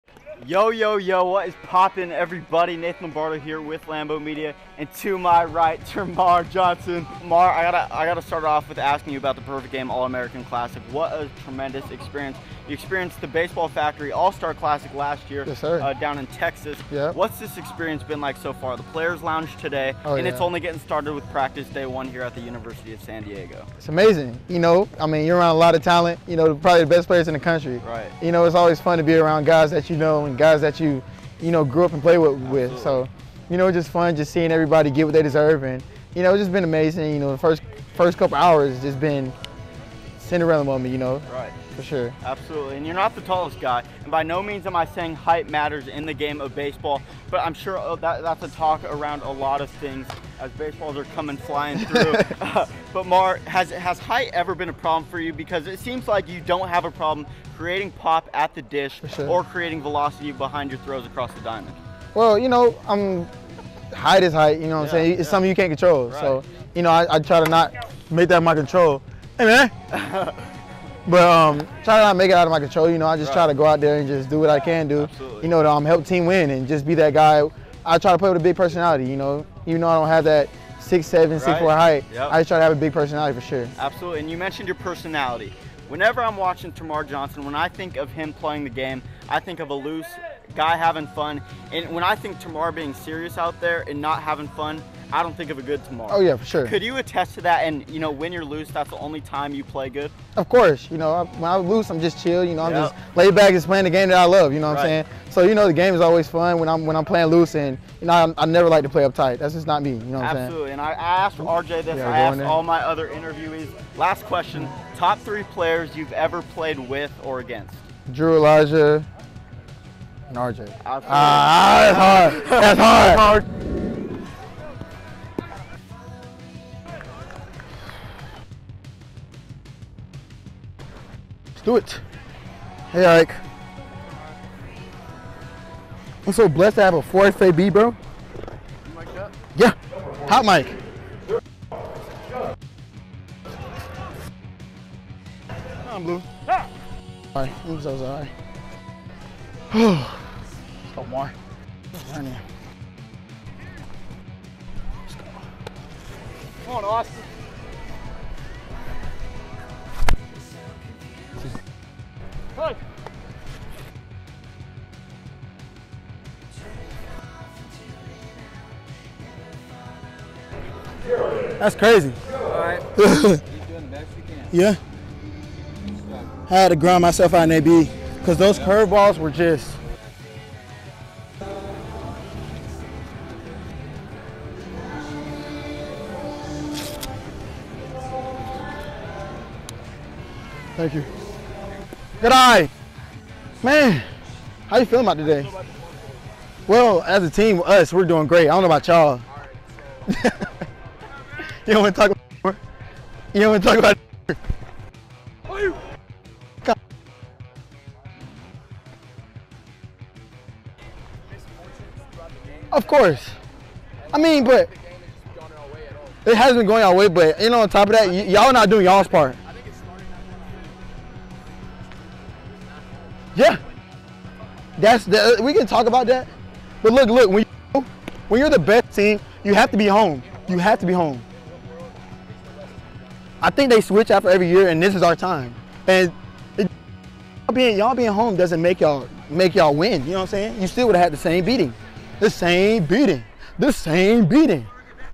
TERMARR JOHNSON MIC’D UP AT THE PGAAC | + AN EXCLUSIVE INTERVIEW
What a segment right here, as Termarr Johnson opens up to me about the haters, the PGAAC experience, and much more. On top of that, the best clips of him mic’d up during an at bat of the Perfect Game All-American Classic.